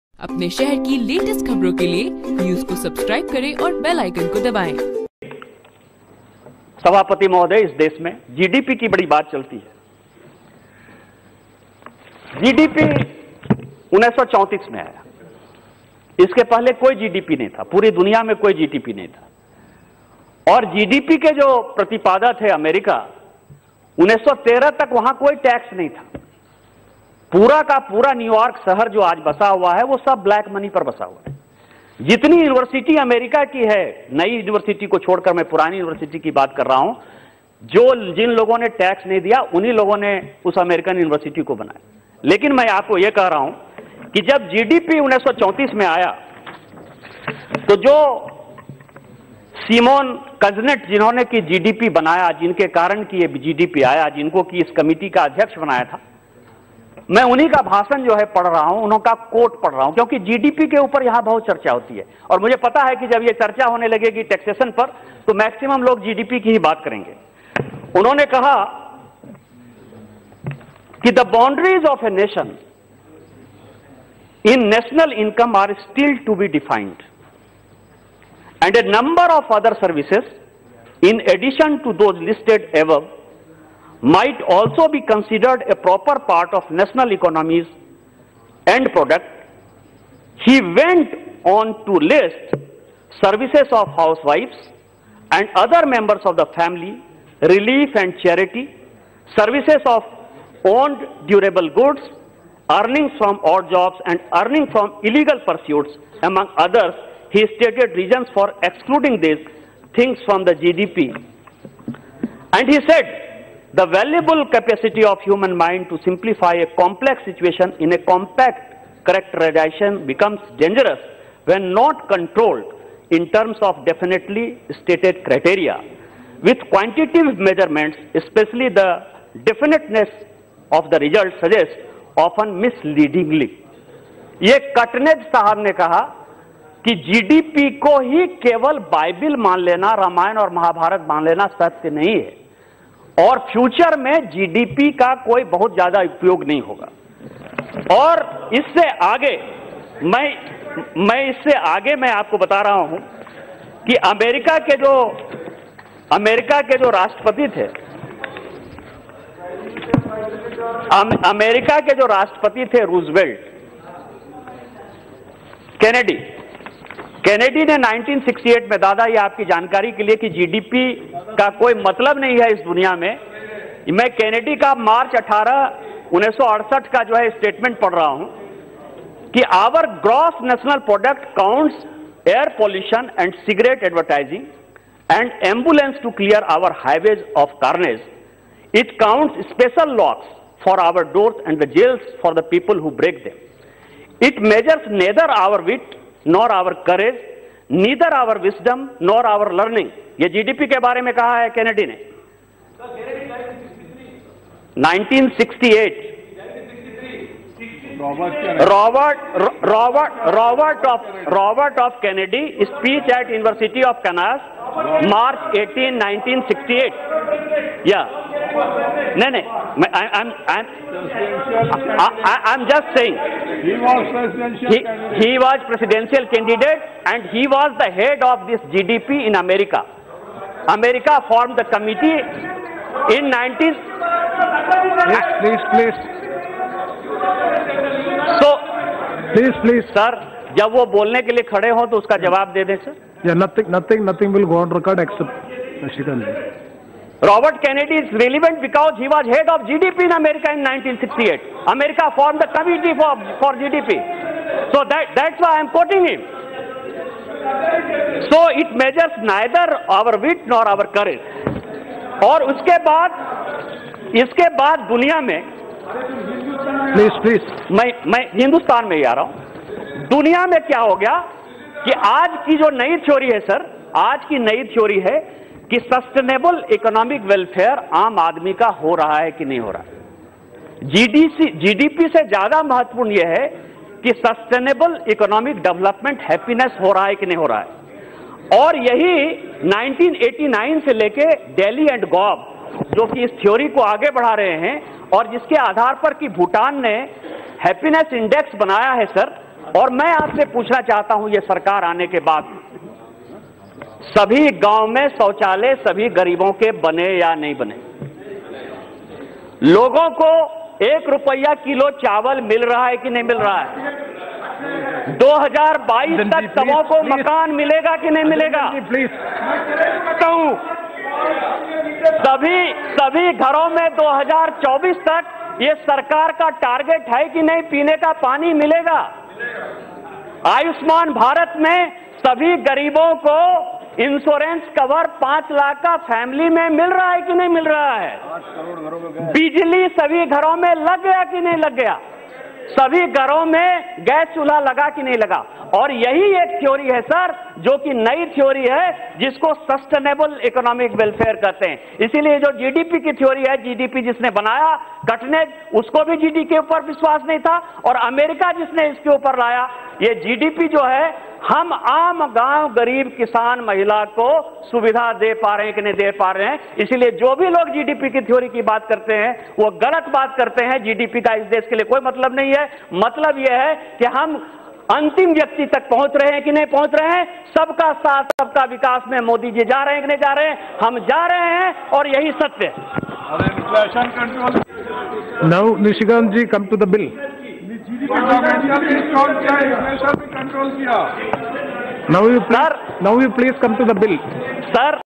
Godda: GDP पर बोले BJP MP Nishikant Dubey- यह बाइबल, रामायण या महाभारत नहीं
संसद के शीतकालीन सत्र का आज 11वां दिन है। संसद में हैदराबाद में महिला पशु चिकित्सक के साथ सामूहिक दुष्कर्म के बाद उसकी निर्मम हत्या कर की गूंज के साथ-साथ कई मुद्दे उठे। संसद में पीएम मोदी और अमित शाह को घुसपैठिया बताने के कांग्रेस सांसद अधीर रंजन चौधरी के बयान का मुद्दा भी उठा। इसके बाद गिरती जीडीपी का मुद्दा भी उठा। जीडीपी के मुद्दे पर लोकसभा में भाजपा सांसद निशिकांत दुबे ने कहा कि जीडीपी 1934 में आई इससे पहले कोई जीडीपी नहीं थी... केवल जीडीपी को बाइबल, रामायण या महाभारत मान लेना सत्य नहीं है और भविष्य में जीडीपी का बहुत ज्यादा उपयोग नहीं होगा।